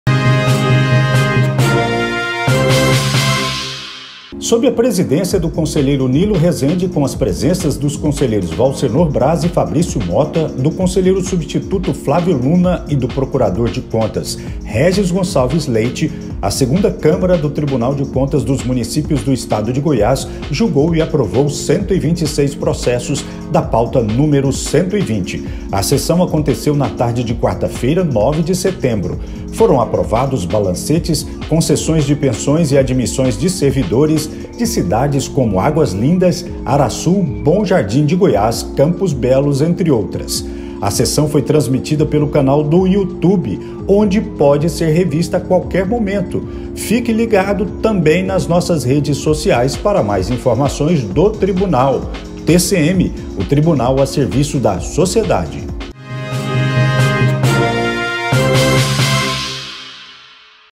Minuto TCMGO - Segunda Câmara - 09/09/20
Sessão remota aconteceu na tarde desta quarta-feira, 9 de setembro, com transmissão pela internet. Mais informações no site do TCMGO.